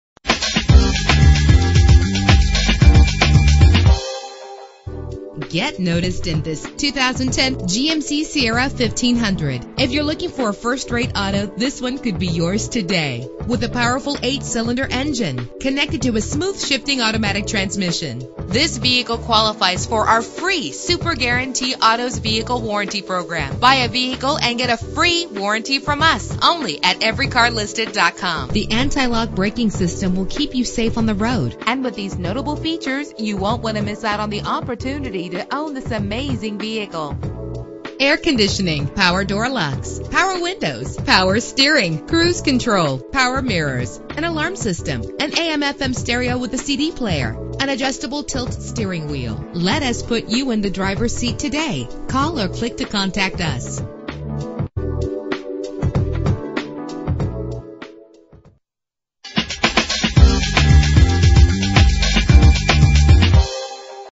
2010 GMC Sierra 1500 - Birmingham AL
Year: 2010
Make: GMC
Model: Sierra 1500
Trim: SL
Engine: 4.8 liter 8 cylinder 16 valve
Transmission: 4-Speed Automatic
Color: Red
Mileage: 10
Address:
1857 Edwards Lake Road